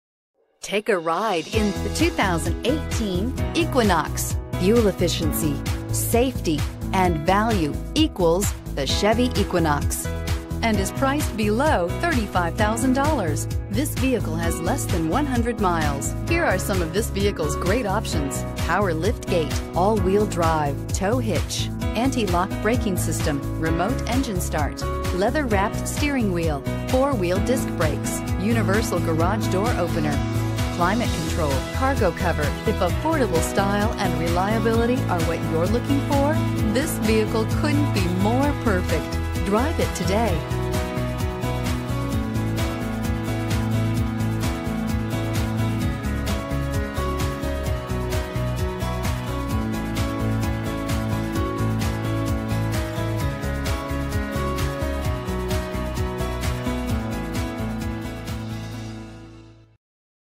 2018 Chevrolet Equinox 00074981
2018 CHEVROLET EQUINOX Sacramento, CA
Stock# 00074981

Performance Chevrolet
4811 Madison Ave.

You'll love this 2018 Chevrolet Equinox. This is a car you'll want to take home. With 12 miles, it features Automatic transmission and an exterior color of Storm Blue Metallic. Call us and be the first to open the car door today!